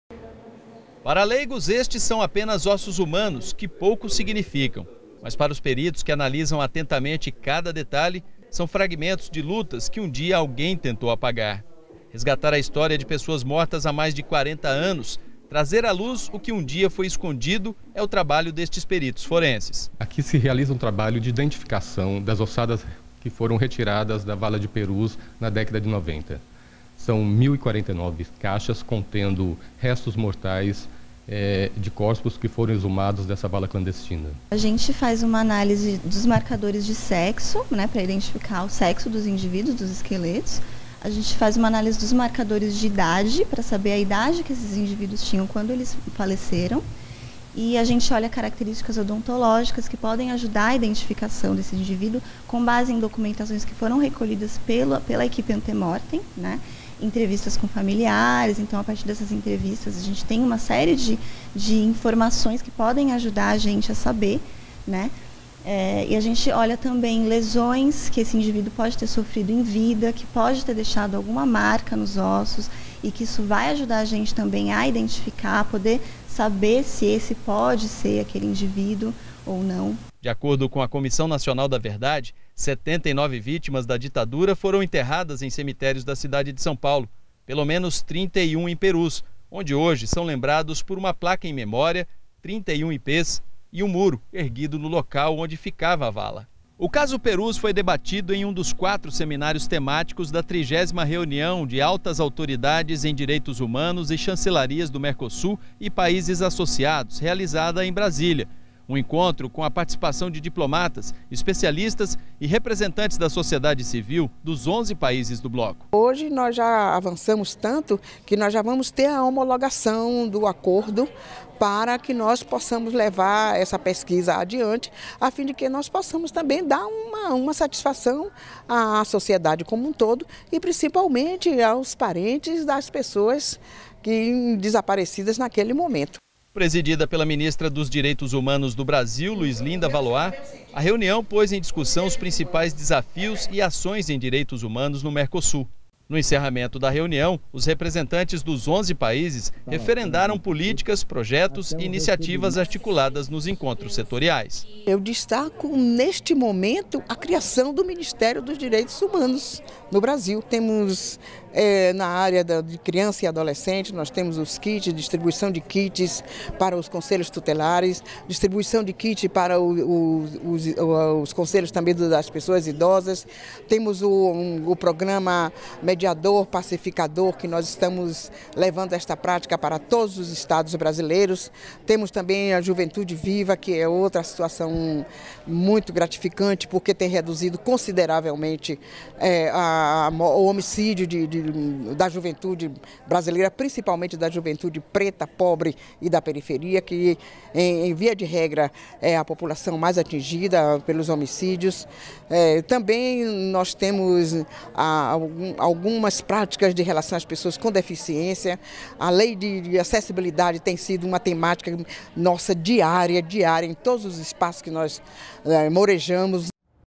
Representantes de países do Mercosul se reuniram em Brasília para discutir Direitos Humanos.
RAAD MERCOSUL - 27.10.17: Representantes de países do Mercosul se reuniram em Brasília para discutir Direitos Humanos. A ideia do encontro é a troca de informações sobre ações e políticas bem sucedidas, desenvolvidas pelos países do bloco.